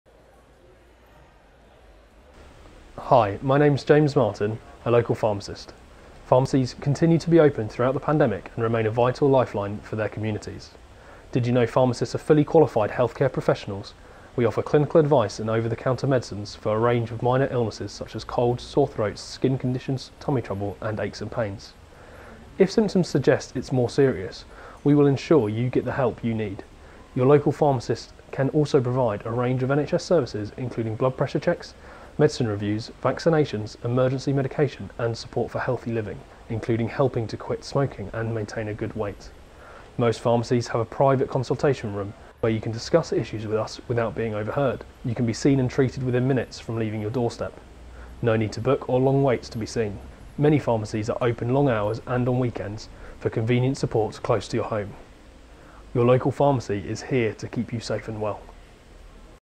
Pharmacy video
Local pharmacists are trained medical professional who can provide free advice on the best treatment for a wide range of illnesses and minor ailments.
Many people are unaware of the wide range of services available from their local pharmacist. Watch our video for information.